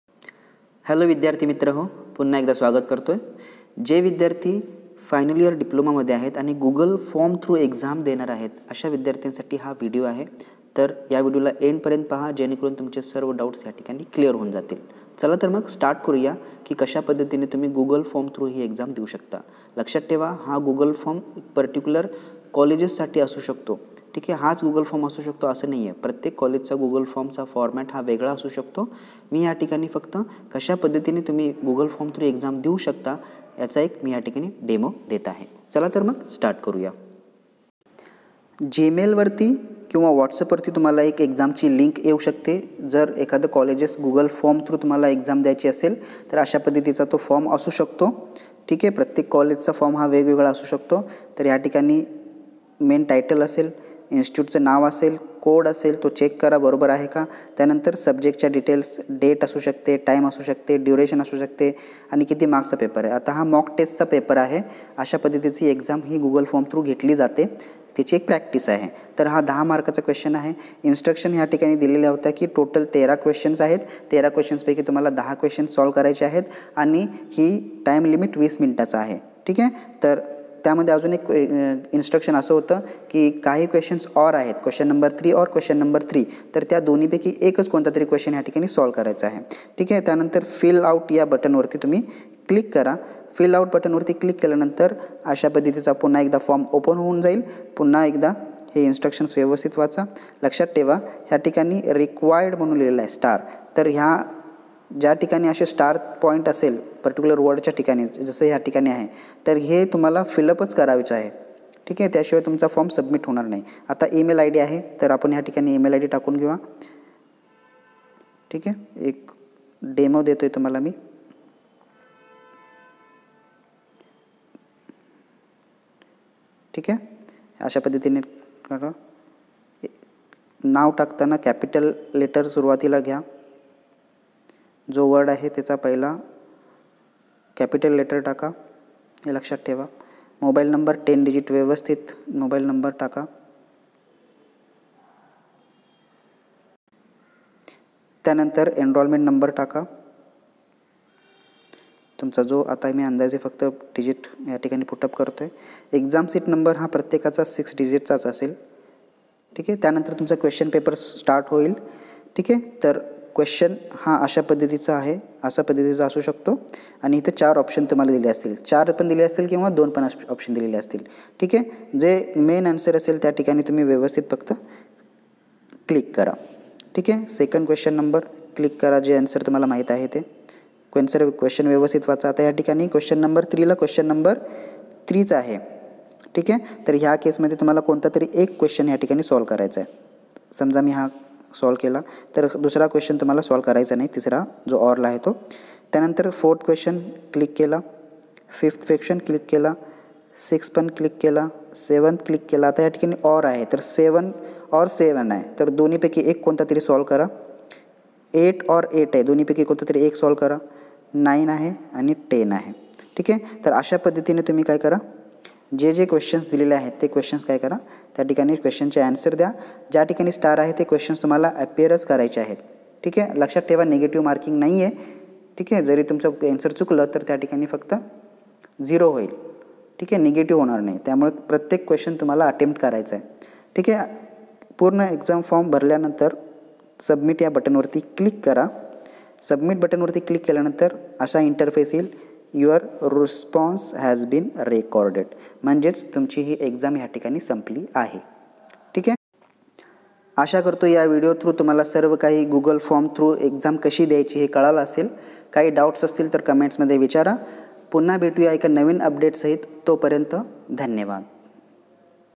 Demo - Diploma Final Year Backlog Exam | MSBTE | How to appear exam through Google Form?
In this video, You can learn how to give the diploma final year backlog exam.

Demo - Diploma Final Year Backlog Exam
How to appear exam through Google Form?